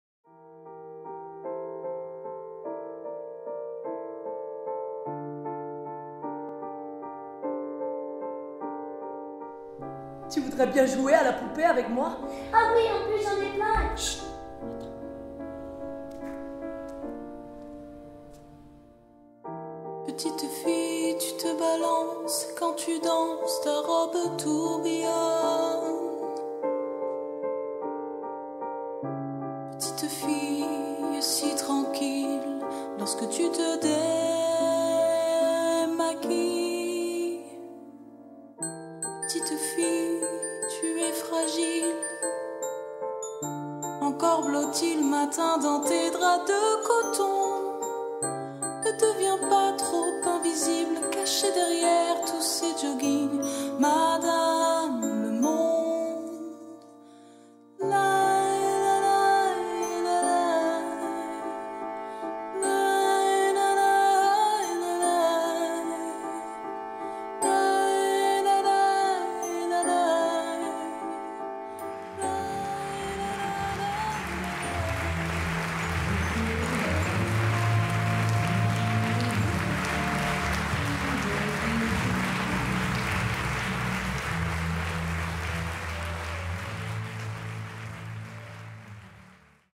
Bande annonce - LES CHATOUILLES ou la danse de la colère - Petit Montparnasse
L’histoire insolite d'Odette, une jeune danseuse dont l'enfance a été volée et qui se bat pour se reconstruire ...

Découvrez toutes les vidéos des spectacles et loisirs de votre région